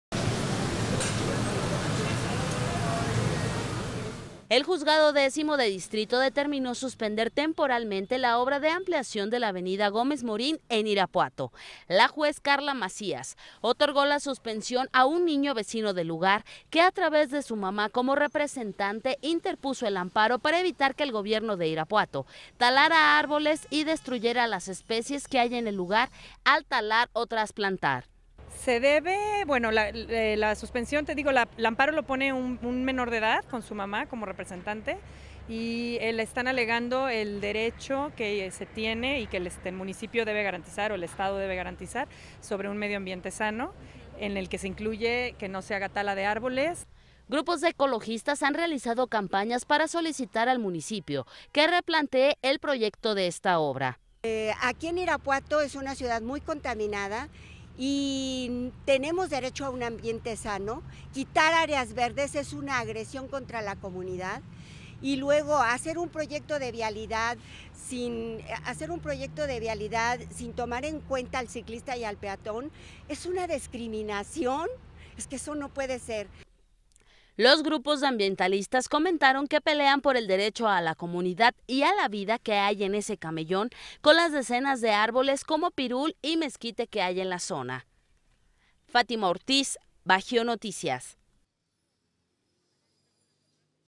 Suspenden obra de ampliación en Irapuato
Un juez de distrito ordenó la suspensión de la obra de ampliación de la avenida Gómez Morin en Irapuato ante el amparo interpuesto por un niño